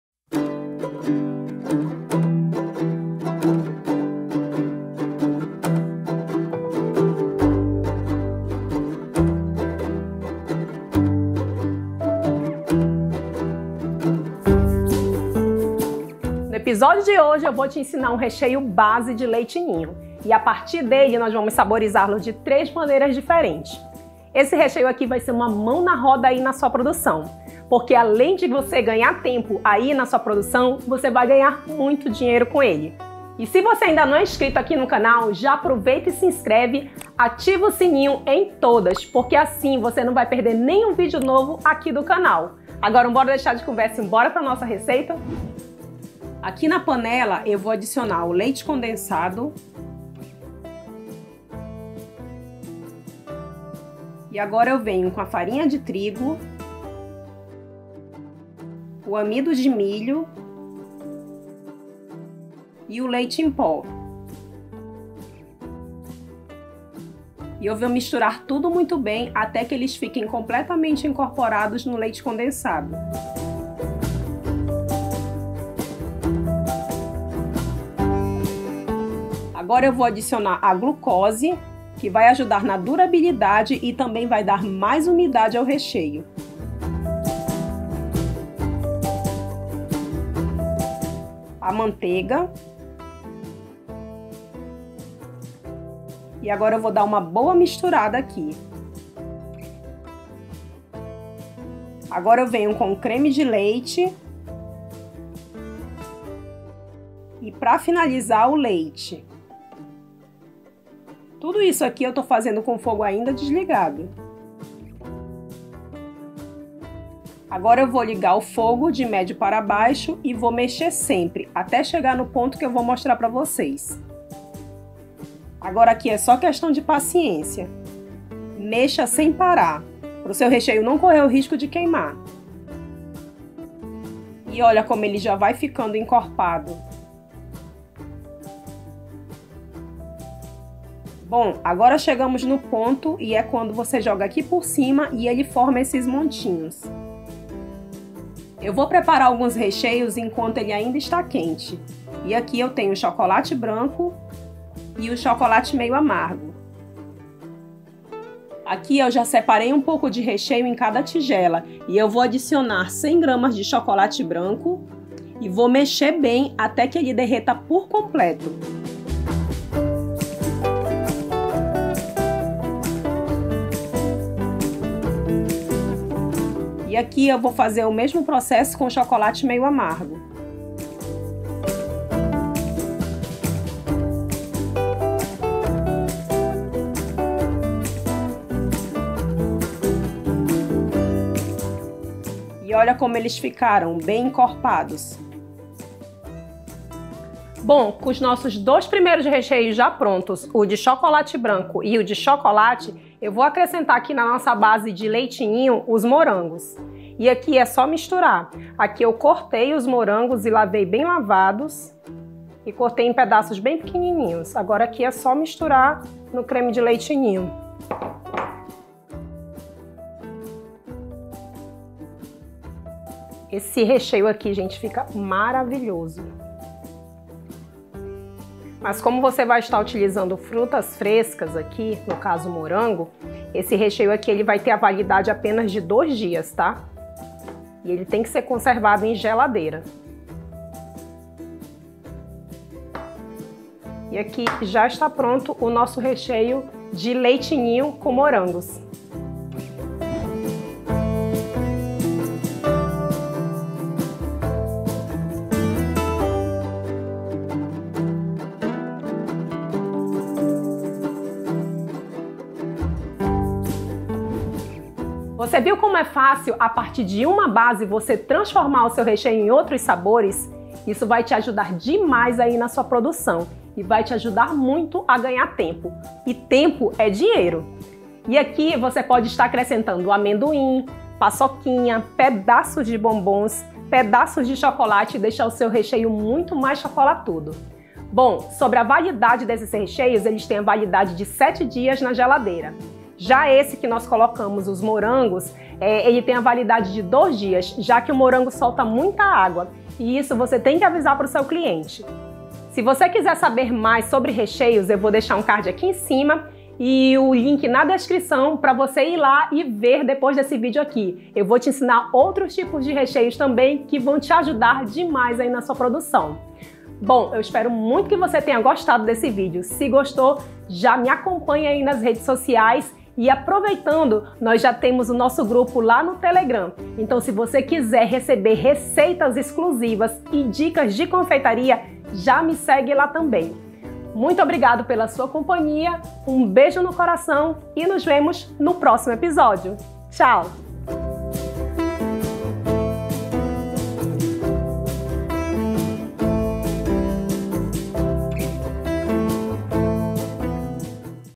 Recheio de leite ninho e + 3 saborizações - Daniele Solem
Hoje eu vou te ensinar o recheio de ninho e mais 3 maneiras diferentes de você saborizar, eles ficam cremosos e maravilhosos!

Esses recheios vão te ajudar a ganhar tempo na cozinha.
Espero que gostem desses recheios para bolos no pote, tortas, bolos e trufas, ovos de páscoa, copos da felicidade. =)

 Para aprender a fazer assistir o vídeo.